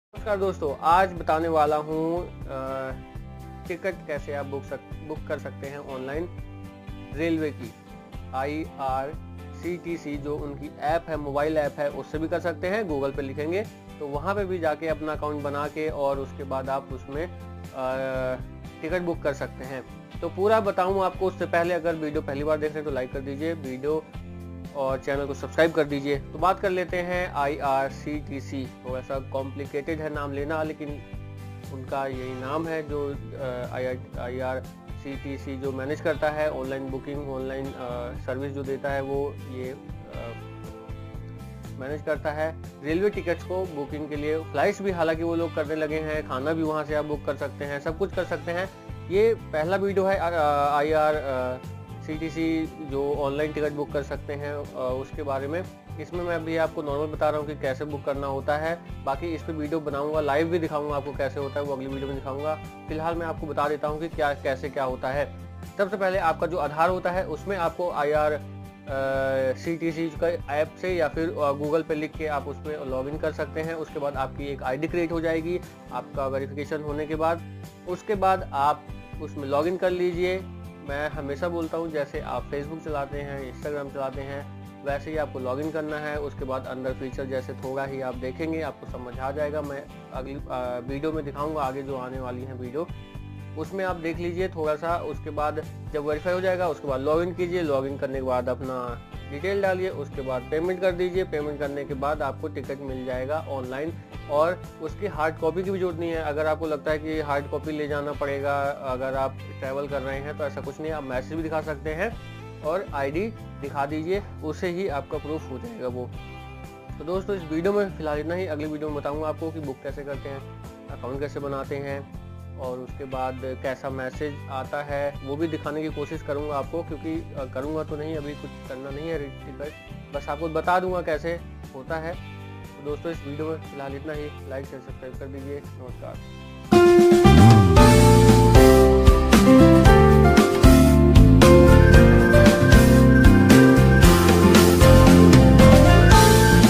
What is IRCTC | How To Use IRCTC | Online Railway Ticket Booking
hello doston es video me apko bataya h ki kaise aap online railway ticket book kar sakte ho.